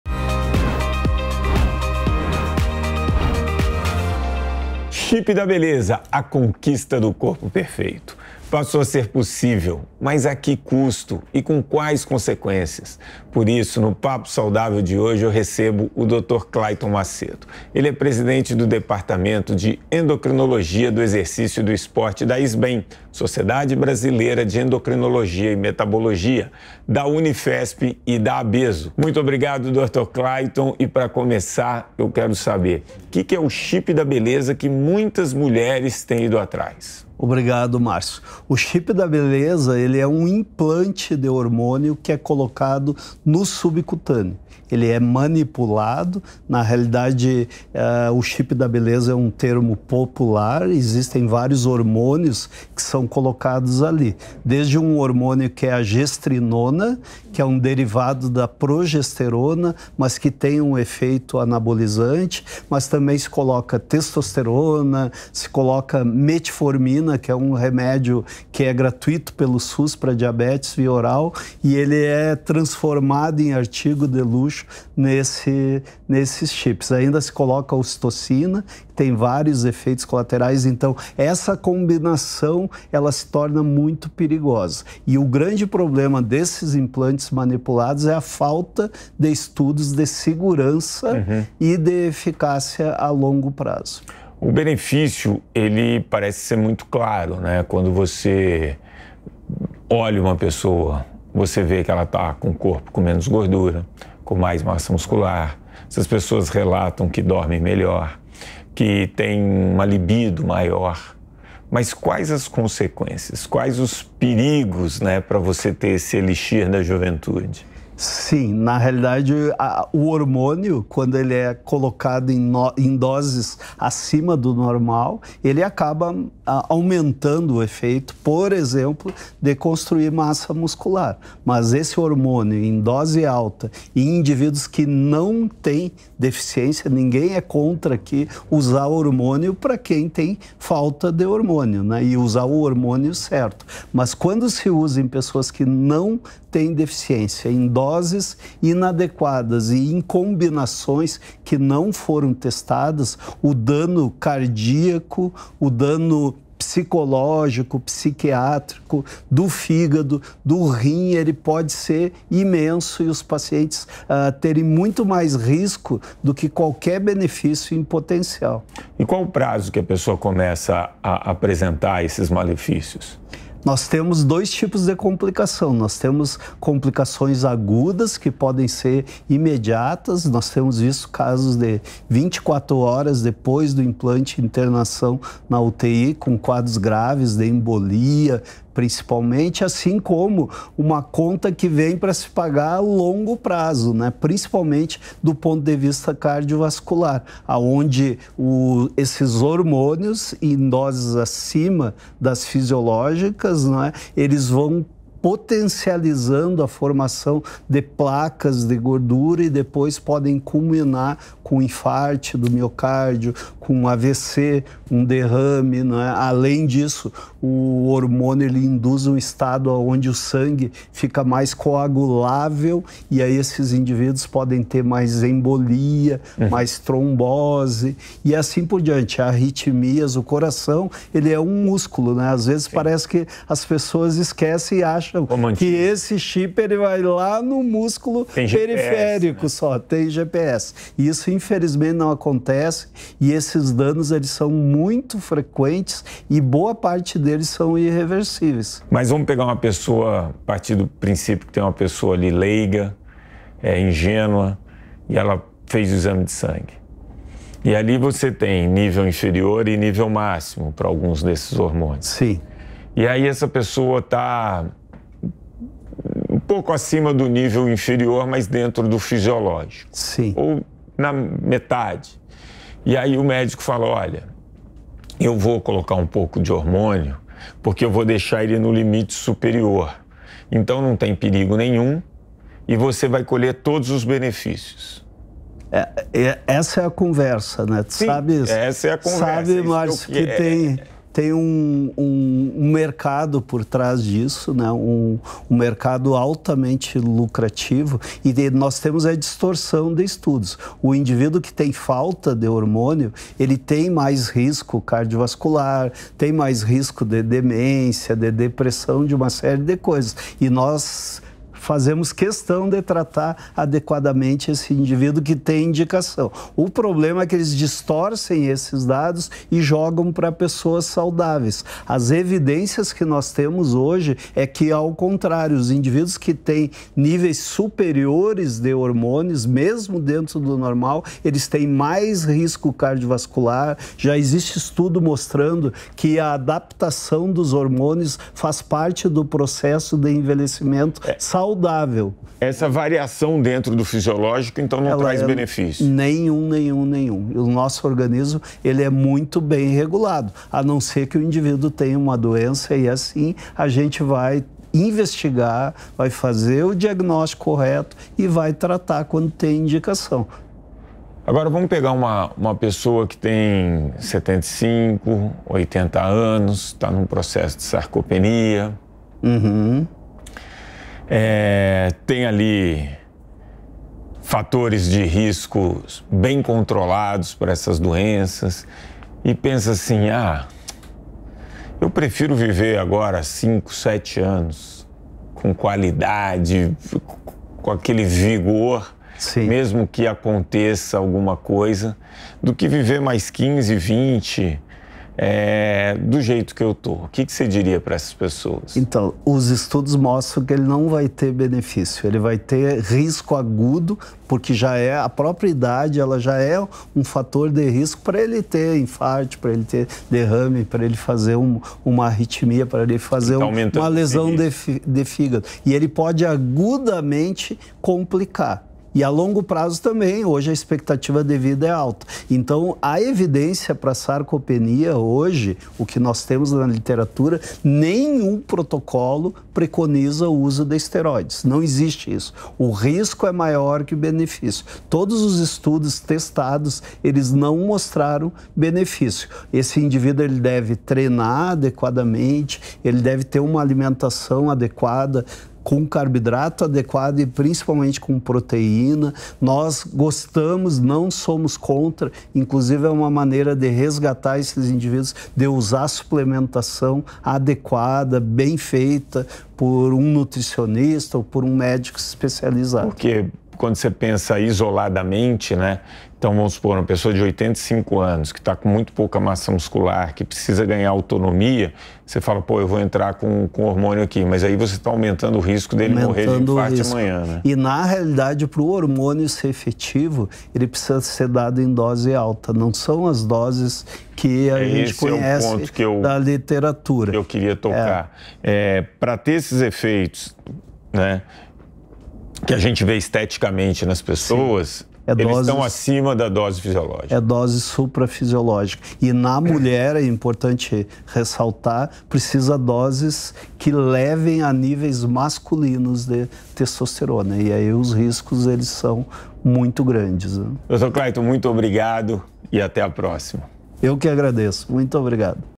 Conheça tudo sobre o famoso ‘chip da beleza’ | VIVA BEM
Conhecido como ‘chip da beleza’, o implante hormonal subcutâneo tem sido procurado por muitas mulheres e com a repercussão, também surgem muitas dúvidas do que se trata o método. Para explicar com mais detalhes, o endocrinologista Clayton Macedo conversou com Marcio Atalla no Papo Saudável desta semana. Confira!